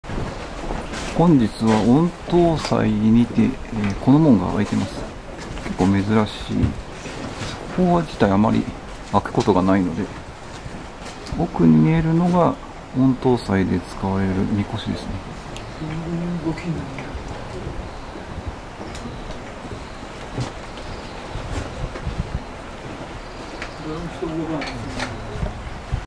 諏訪大社 御頭祭（上社例大祭）神輿と 四脚門
実際居移動する際に使われるお神輿と
 四脚門は神事がなければ常に閉まっているので
珍しい光景ではあります

20～30万以上神社があります。　実はコンビ二よりも多い